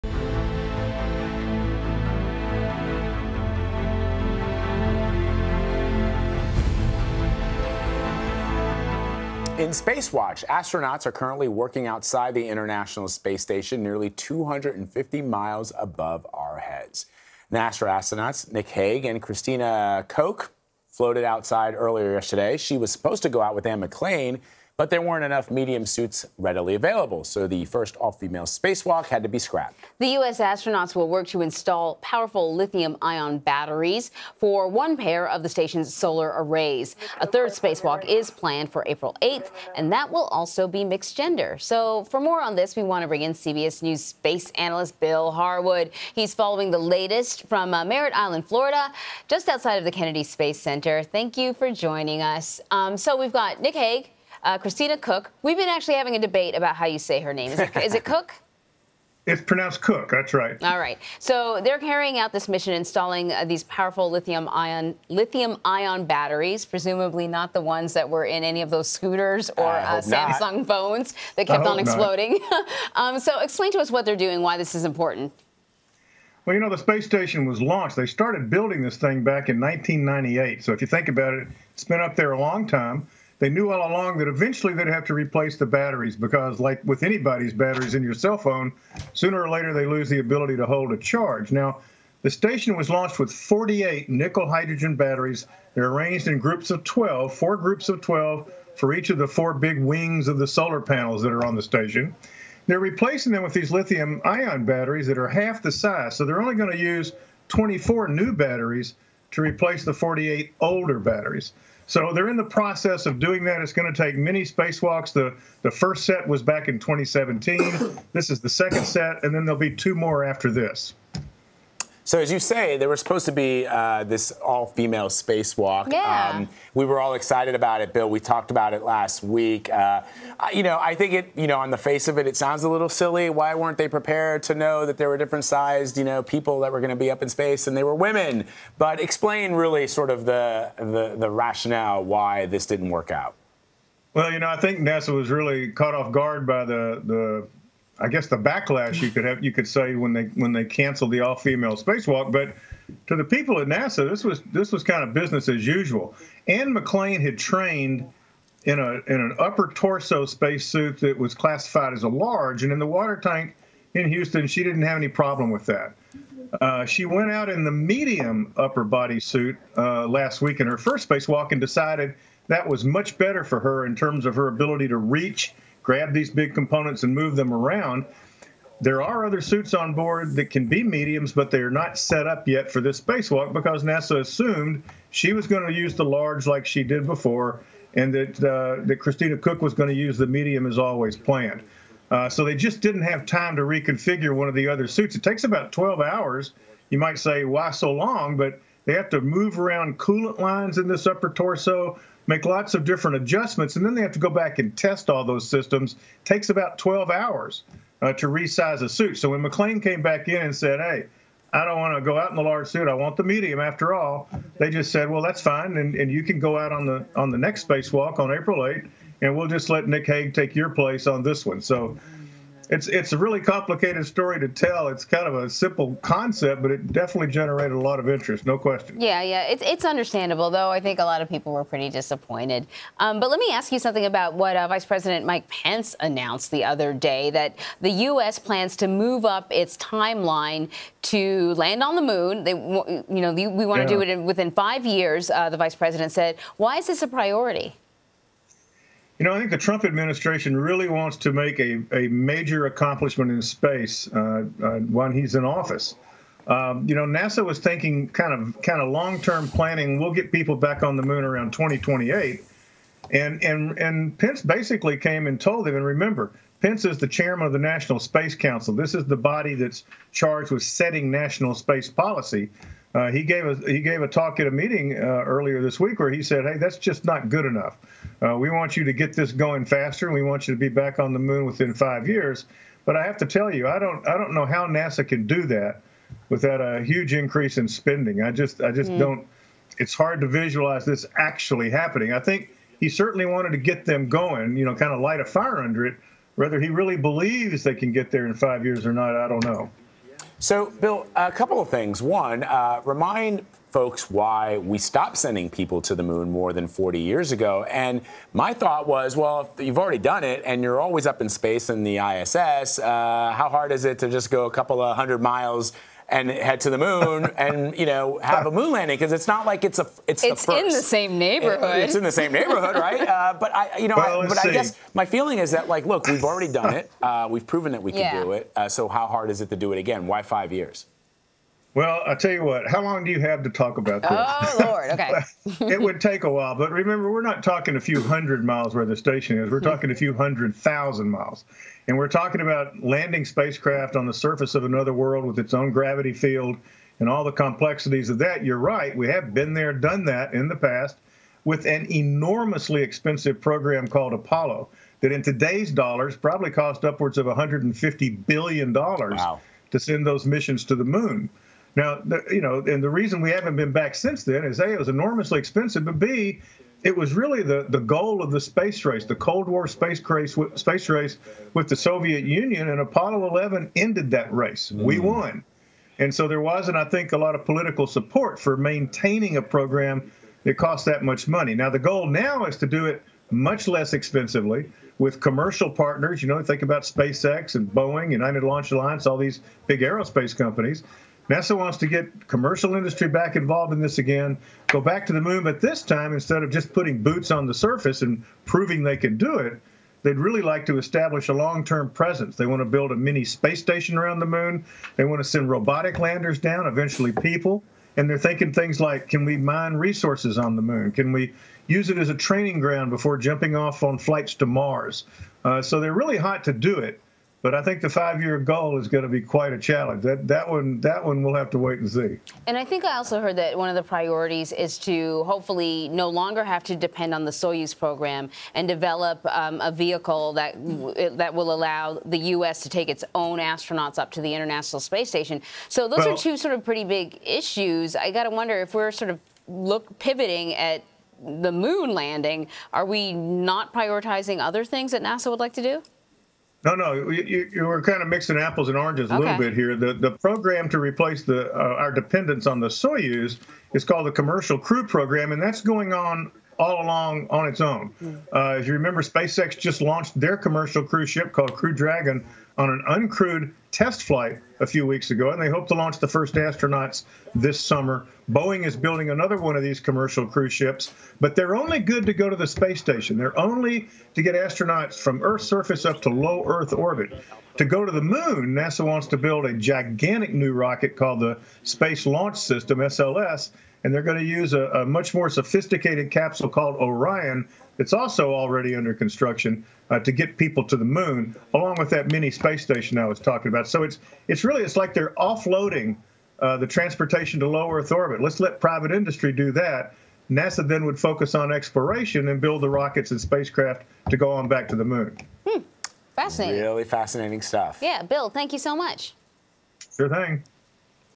NASA astronauts on second spacewalk mission of 2019
CBS News space analyst Bill Harwood joins CBSN with insight on NASA's second spacewalk of the year at the International Space Station, and what could soon be a space race to the moon.